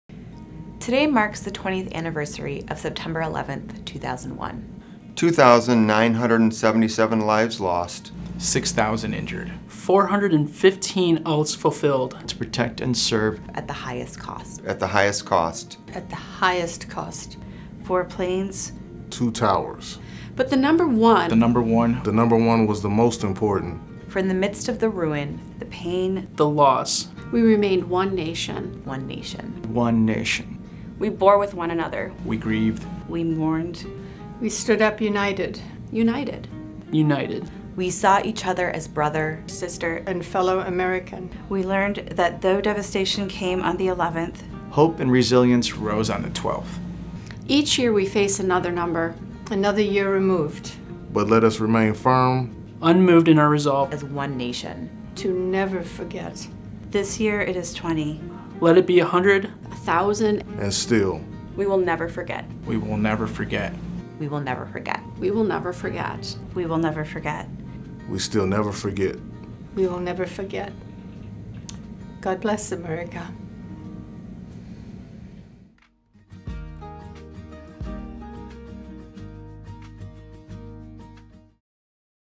We will never forget.
It is a solemn day of remembrance as we mark the 20th anniversary of 9/11.

Despite our shared grief in the aftermath, we will never forget the hope, resilience, and unity with which our nation responded.